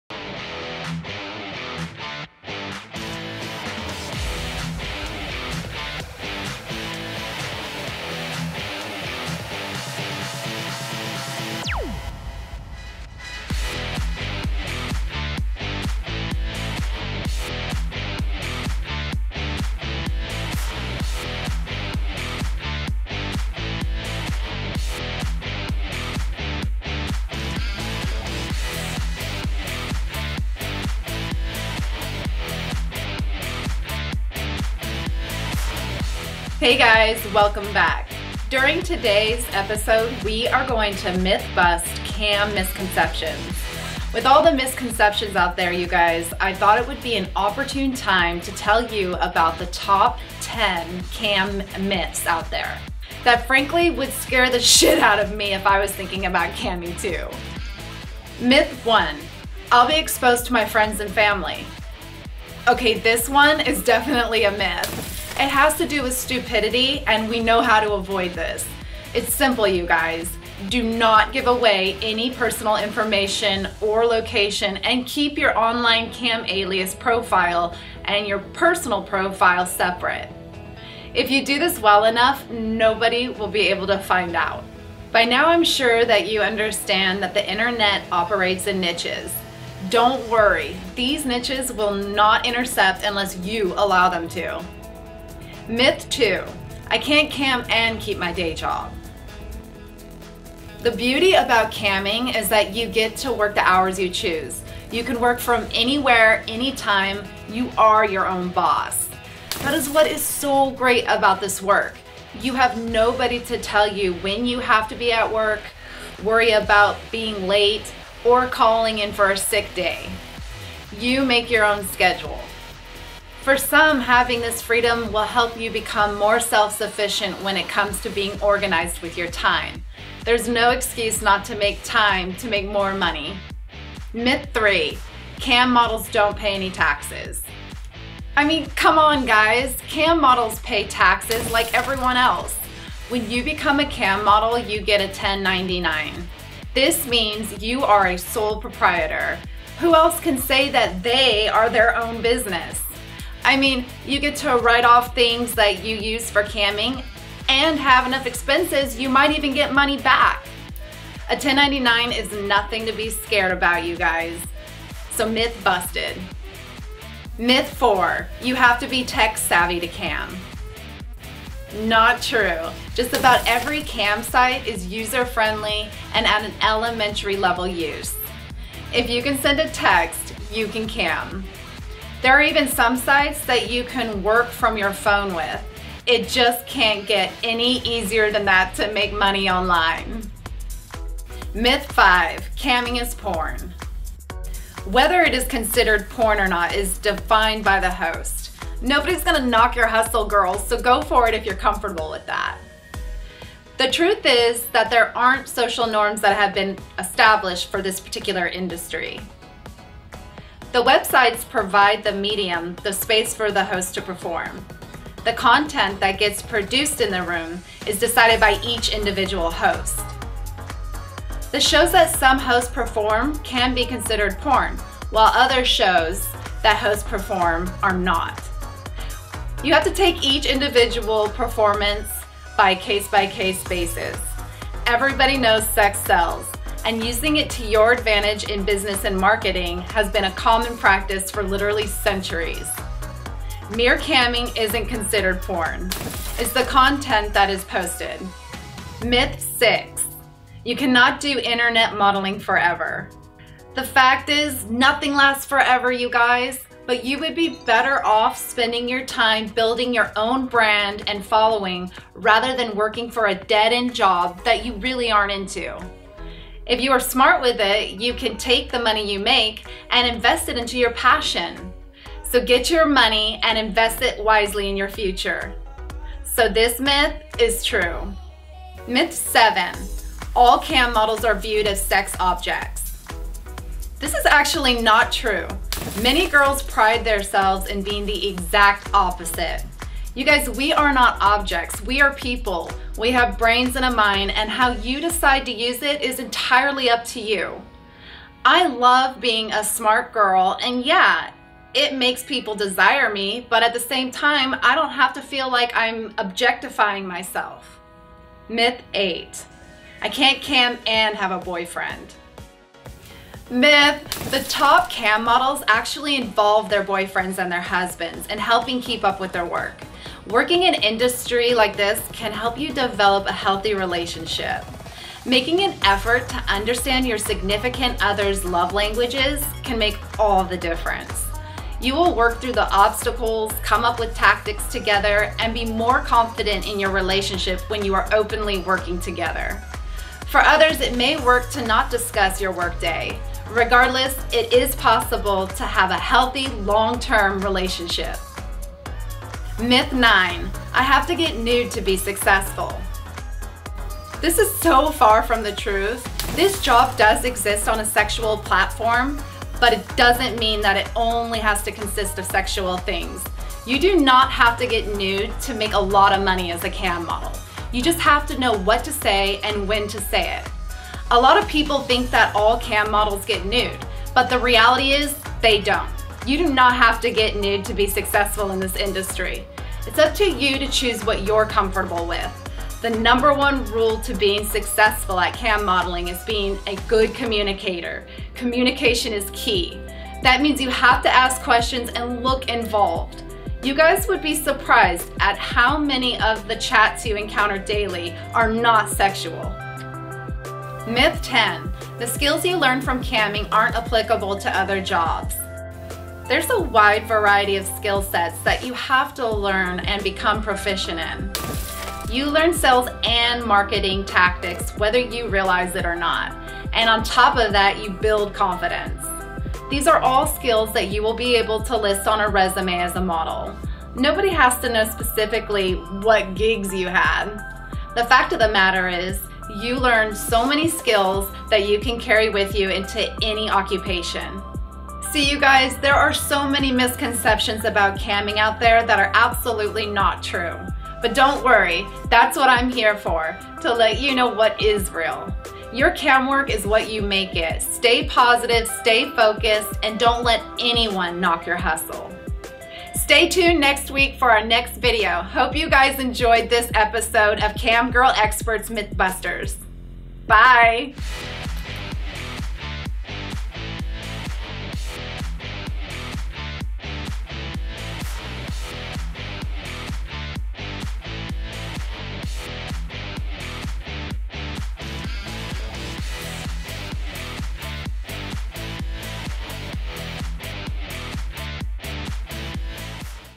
CamGirl Expert S1 Ep5: Busting Myths About Webcam Jobs
Common camming myths: true or not?
 CamGirl Expert will tell you!